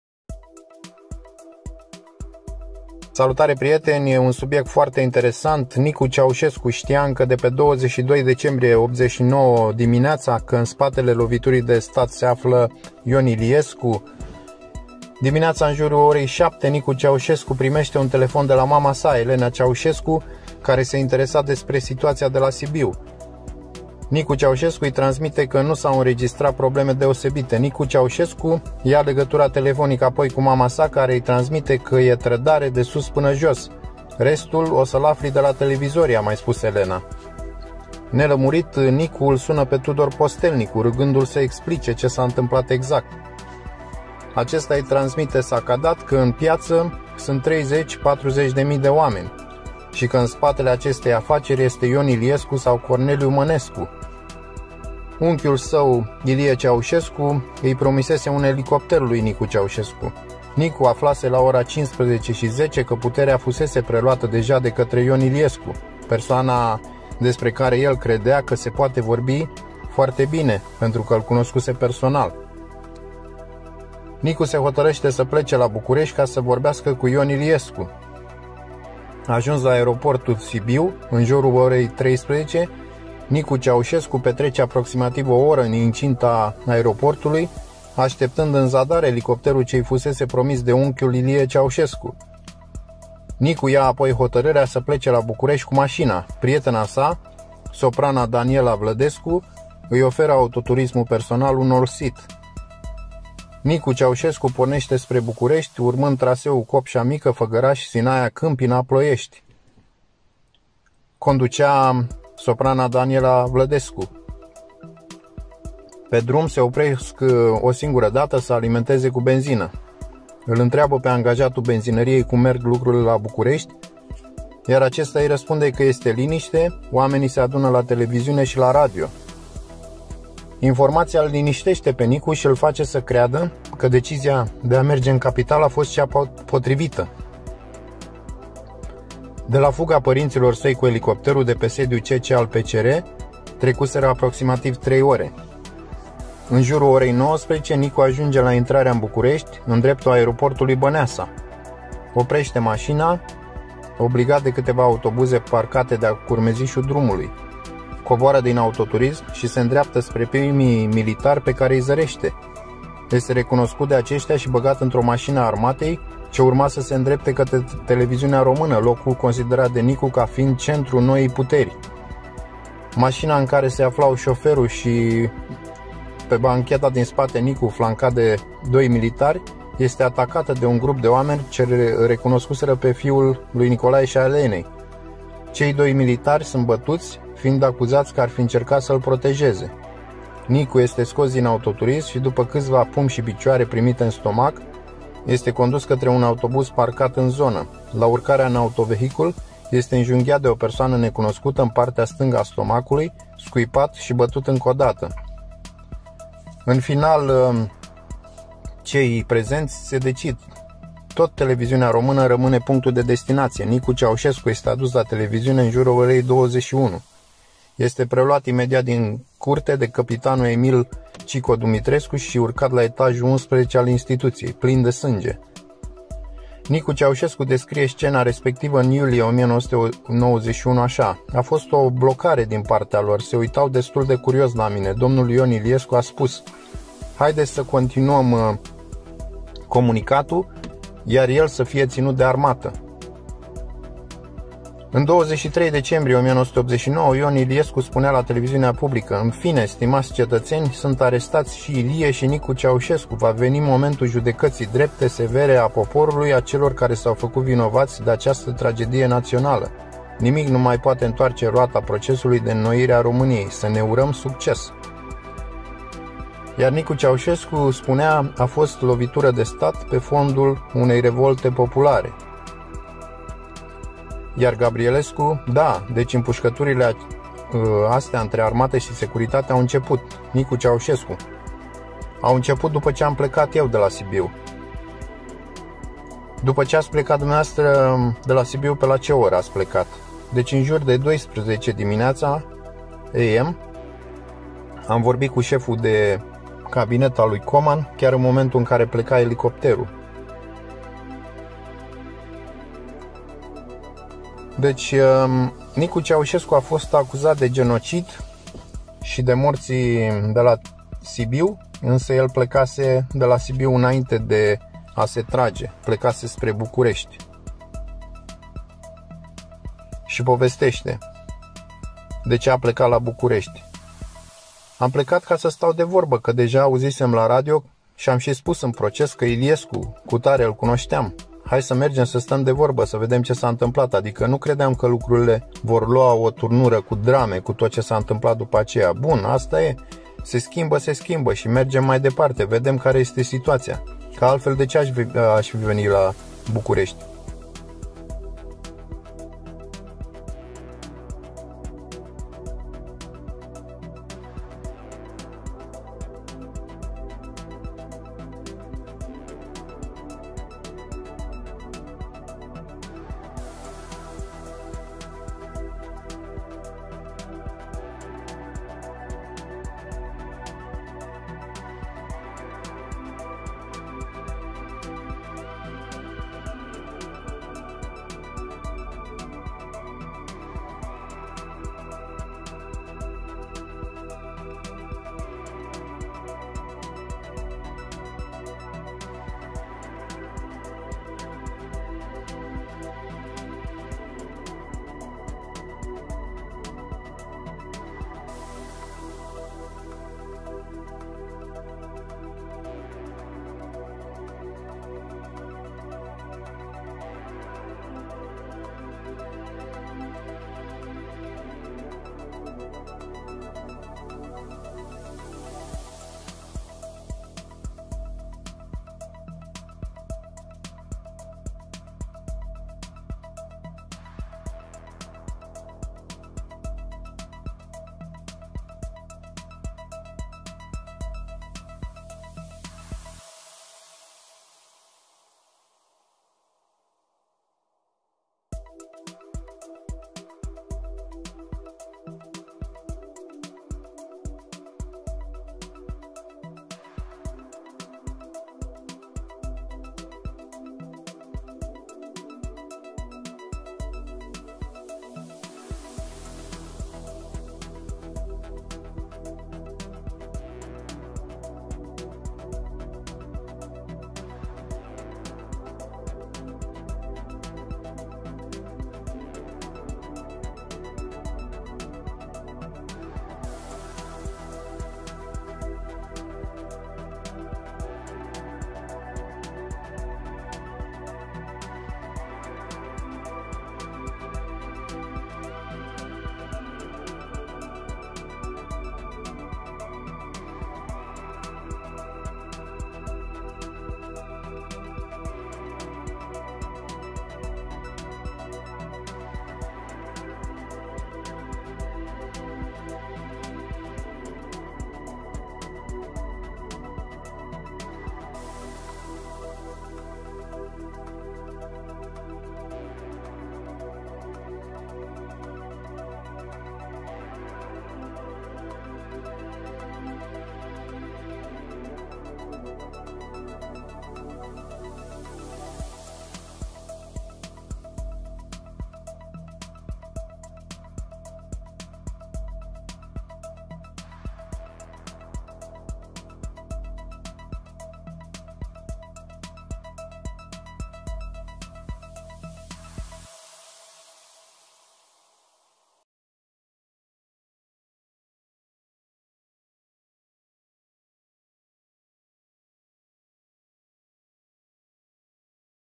Nicu Ceaușescu: "Știam din 22 Decembrie Dimineață că Ion Iliescu e în Spatele Loviturii de Stat"
Nicu Ceaușescu: "Mi s-a Comunicat în 22 Decembrie Dimineață la ora 10 că Ion Iliescu e în Spatele Loviturii de Stat. Lucrurile erau Clare!"